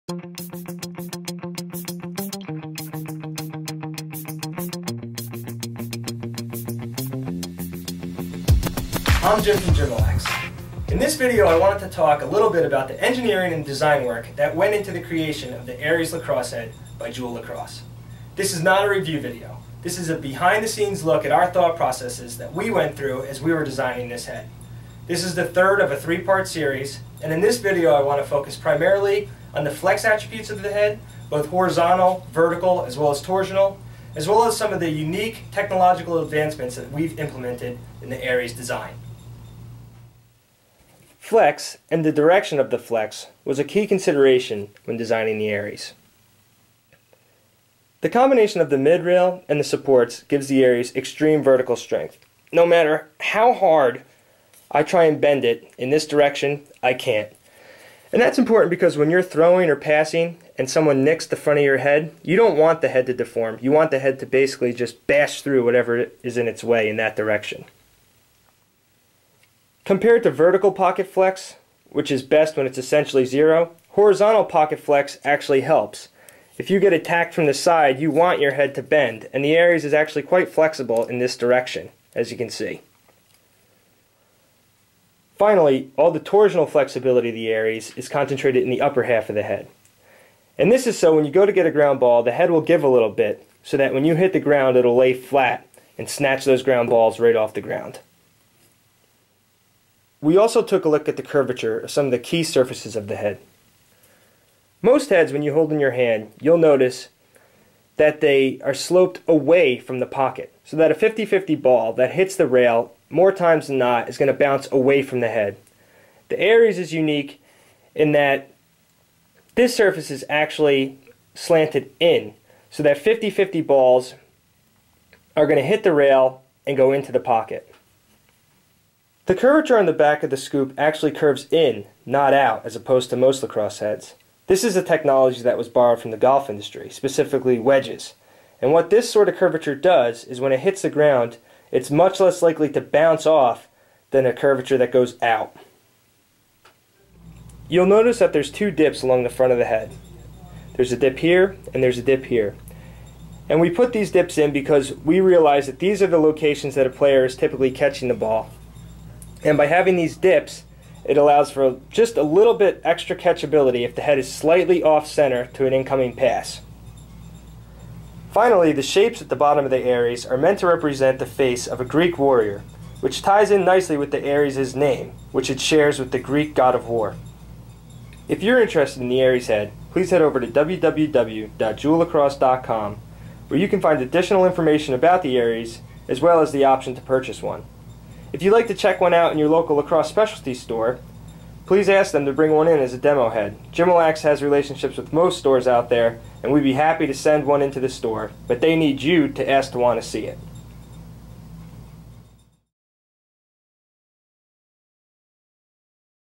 Engineering the Ares - Part 3 - Flex/Technology
Part 3 of a 3 part series investigating the engineering behind the Ares lacrosse head, the newest product from Jimalax's sister company, Joule Lacrosse. For additional information and purchase options, head over